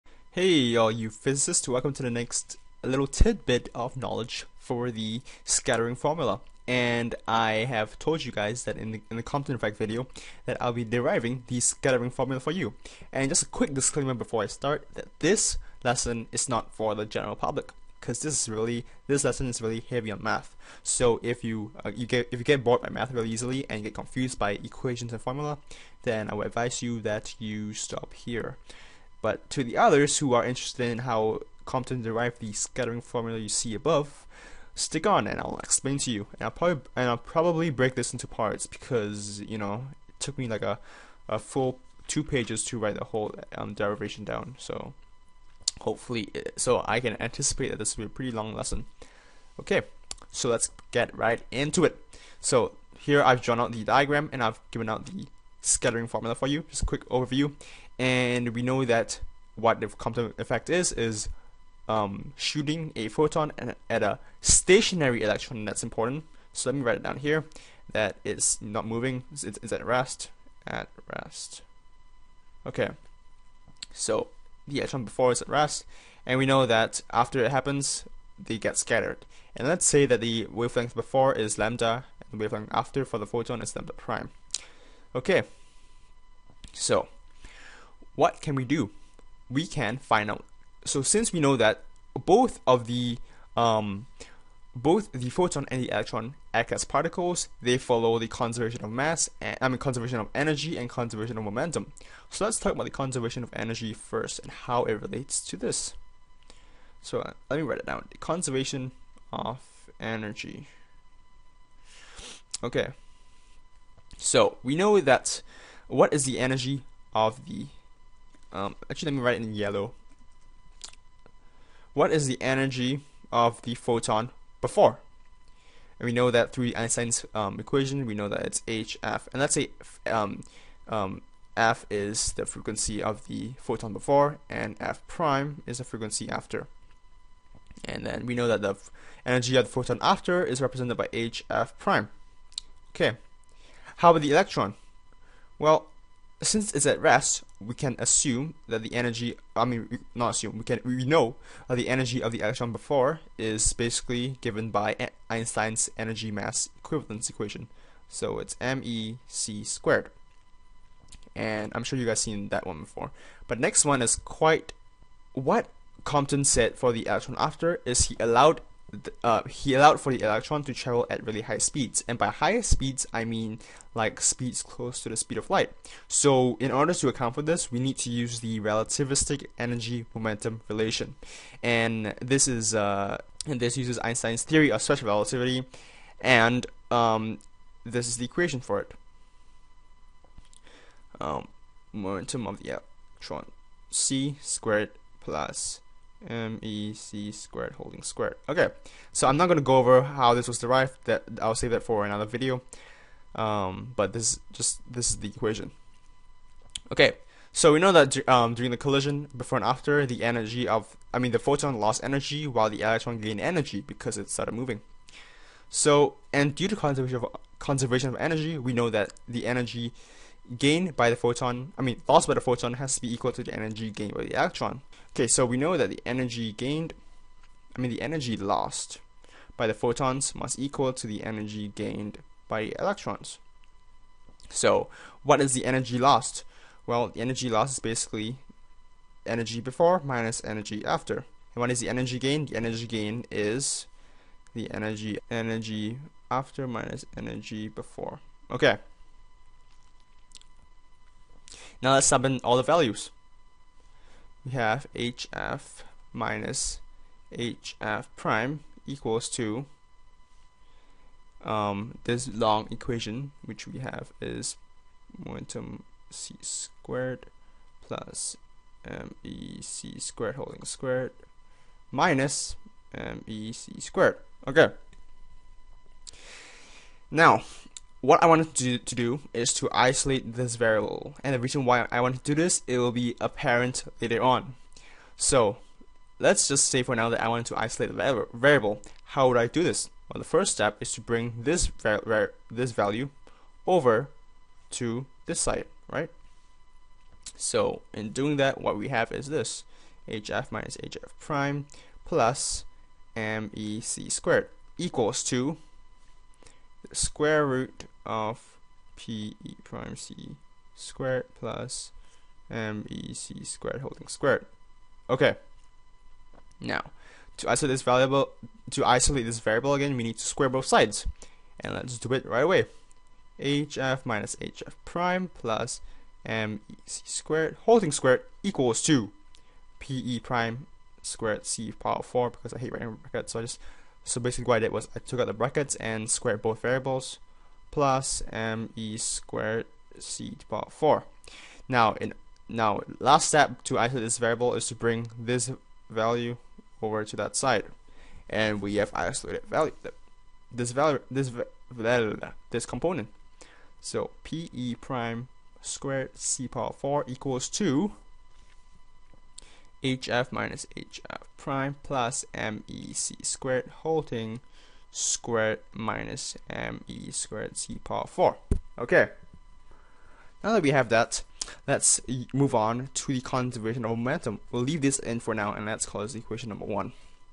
Deriving the Compton Scattering Formula (Part 1)
This is part one of my lesson on the derivation of the scattering formula relating the shift in the photon's wavelength to its scattering angle.  In this part, I go over the calculations involving the conservation of energy.